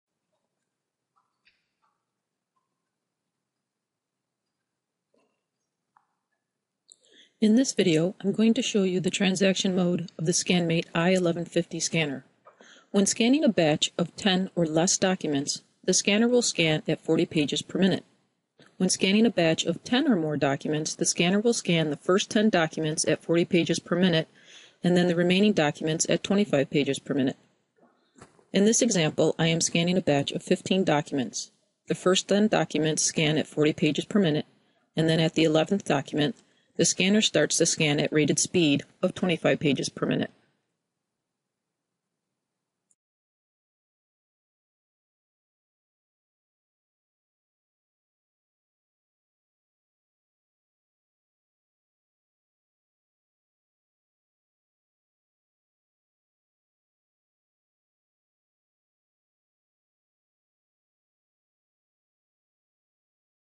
Transaction mode on the Kodak ScanMate i1150 Scanner
This video shows you the transaction mode feature of the Kodak ScanMate i1150 Scanner. The scanner will scan the first 10 documents at 40ppm.